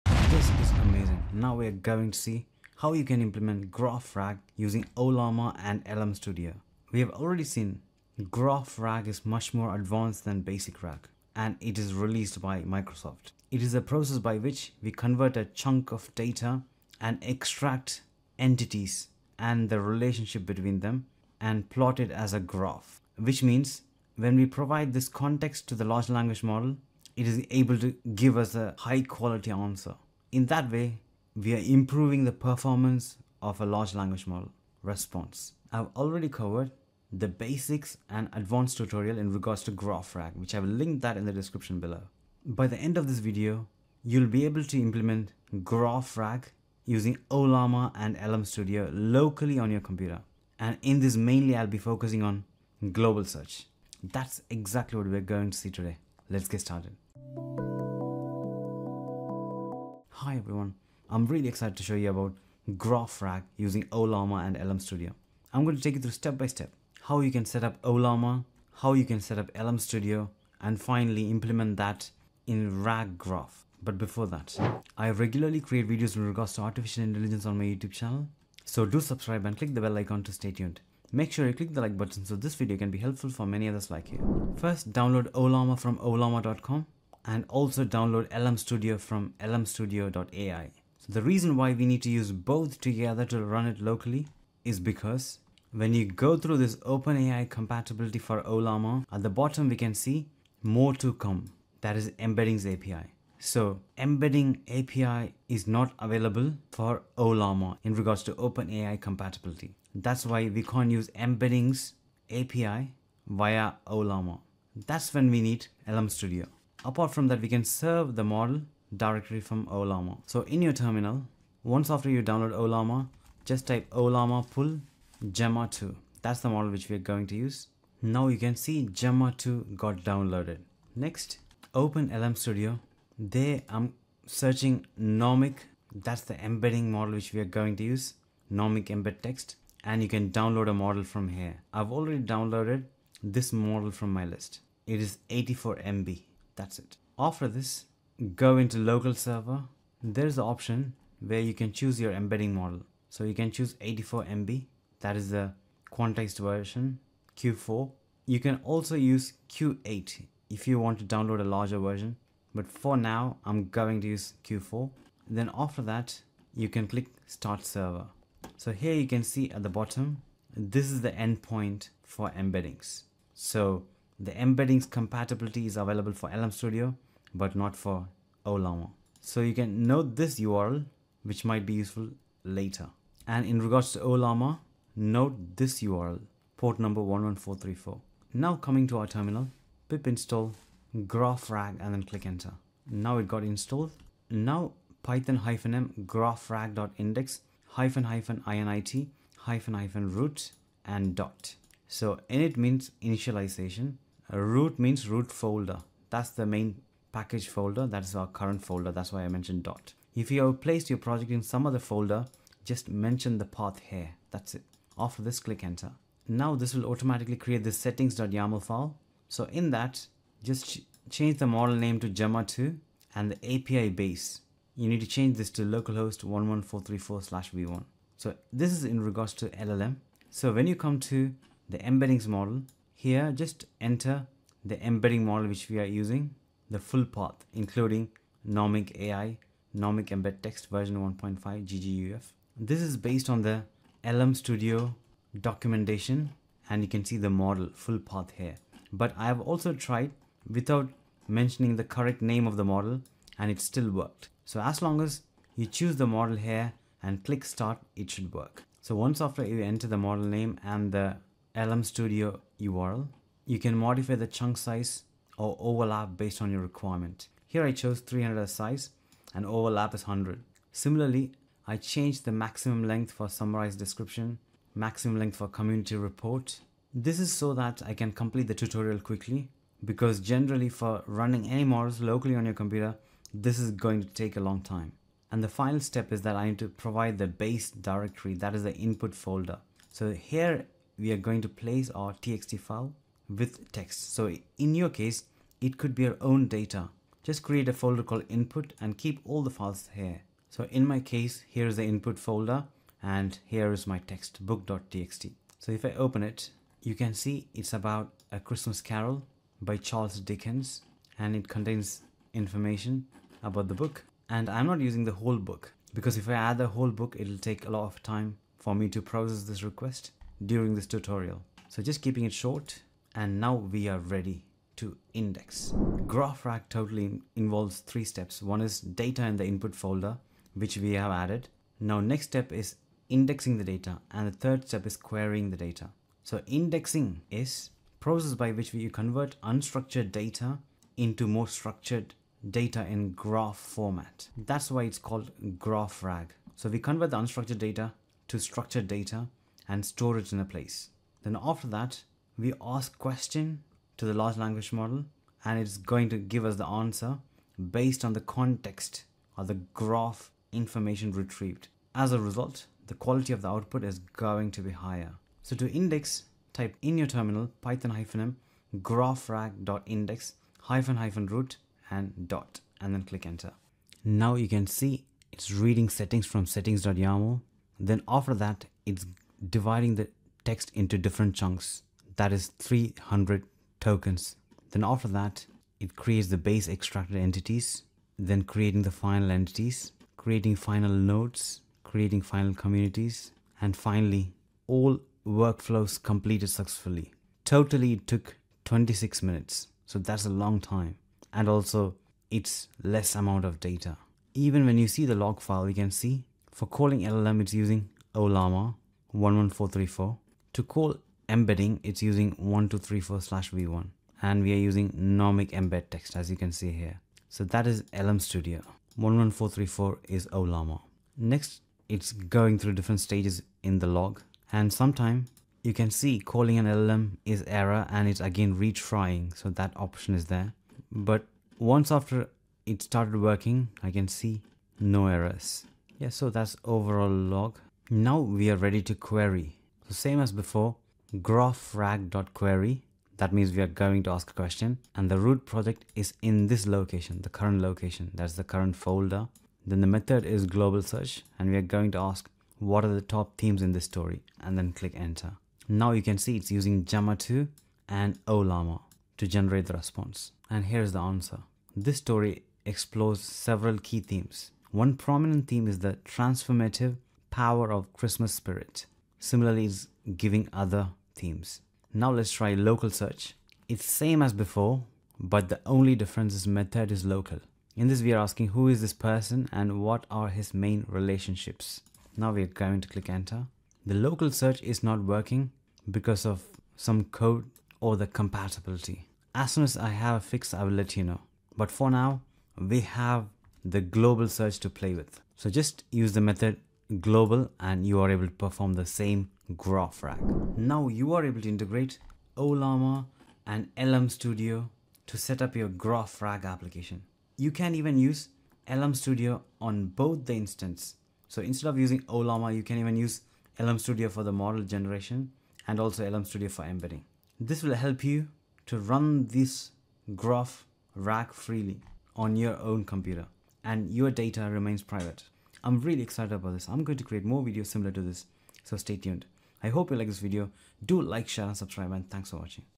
GraphRAG Ollama: 100% Local Setup, Keeping your Data Private
In this video, we'll delve into the world of Graph RAG, demonstrating how to implement it using oLlama and LM Studio. You'll learn how to convert chunks of data into structured entities and relationships, improving the performance of your language model responses. This comprehensive tutorial will guide you through each step, ensuring you can set up and run GraphRAG locally on your computer. 🖥️✨ GraphRAG Ollama: 100% Local Setup, Keeping your Data Private

📚 Topics Covered:
Introduction to GraphRAG and its advantages
Setting up oLlama and LM Studio
Integrating both tools for effective Graph RAG implementation
Running models locally to keep your data private
Troubleshooting and optimising the setup for better performance

📅 Timestamps:
0:00 - Introduction to GraphRAG
1:00 - Downloading and Setting Up Ollama & LM Studio
2:50 - Configuring Models and Embeddings
4:25 - Indexing Data with GraphRAG
7:20 - Querying Data: Global Search
8:20 - Local Search Issue

🔥 Don't Miss Out:
Click the 🔔 bell icon to stay updated with our latest videos!